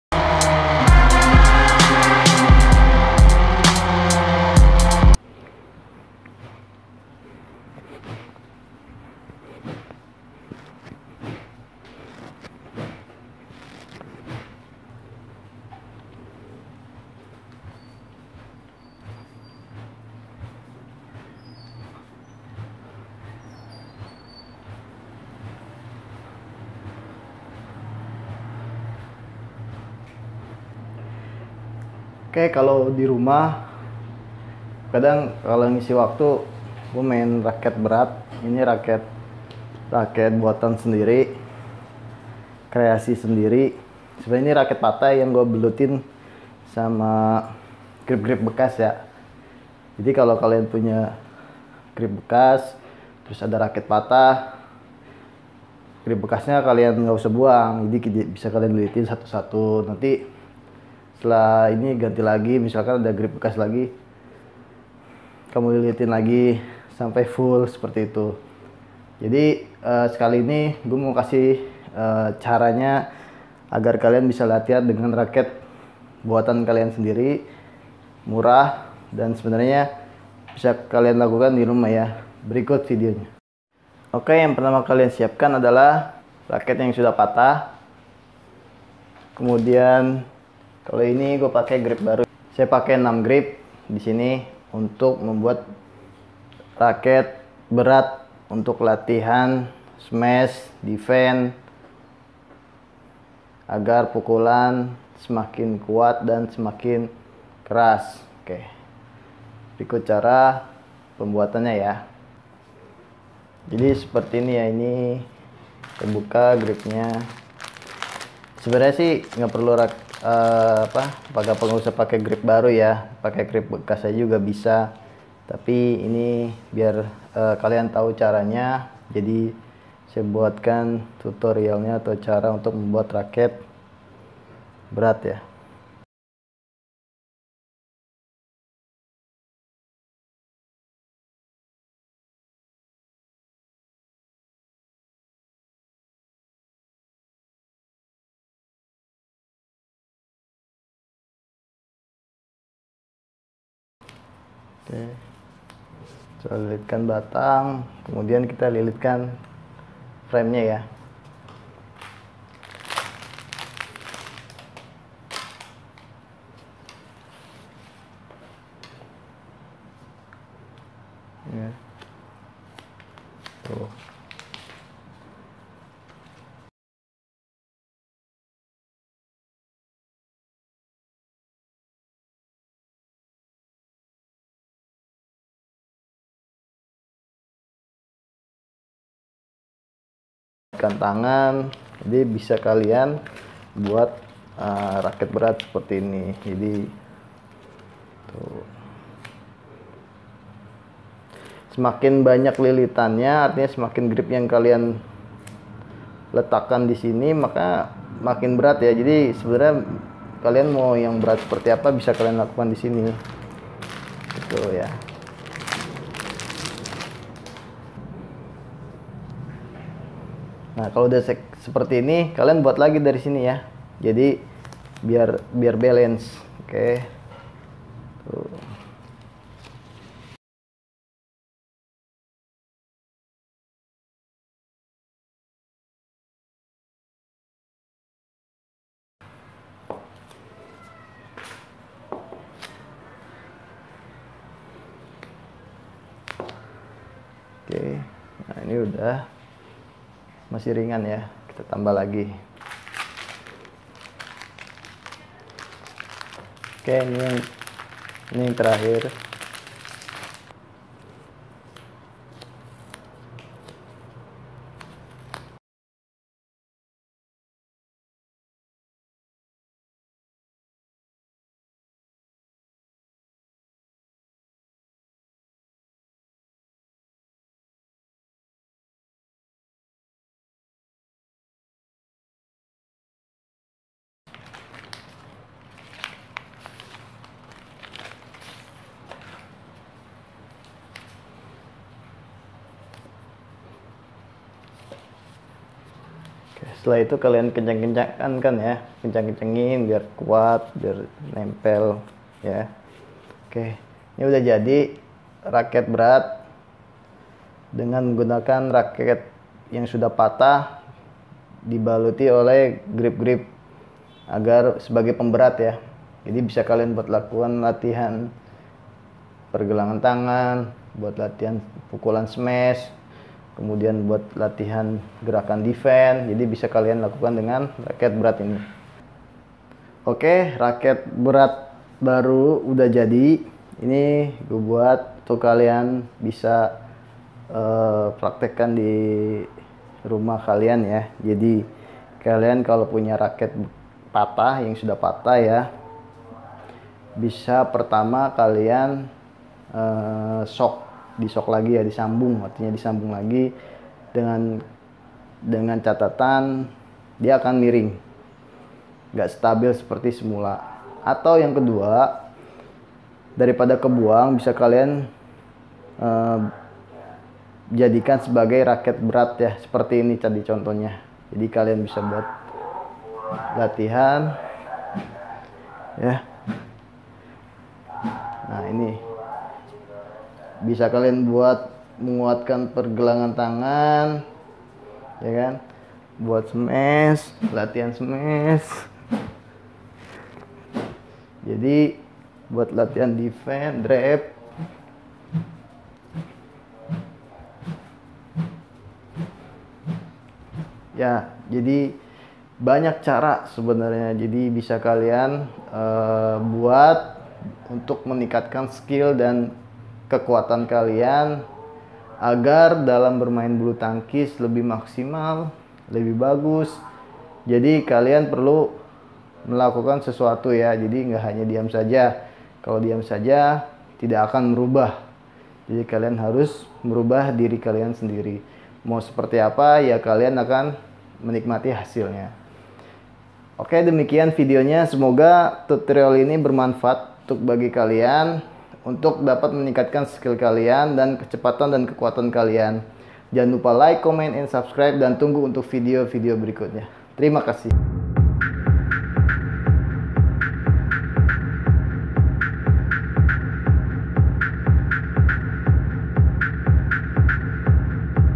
CARA BUAT RAKET BERAT UNTUK LATIHAN KEKUATAN SMASH
Siapkan:
1. Raket yg suda patah/bekas
2. Grip handuk bekas / baru

Ikuti sesuai video

Terima kasih
Mpin Bultang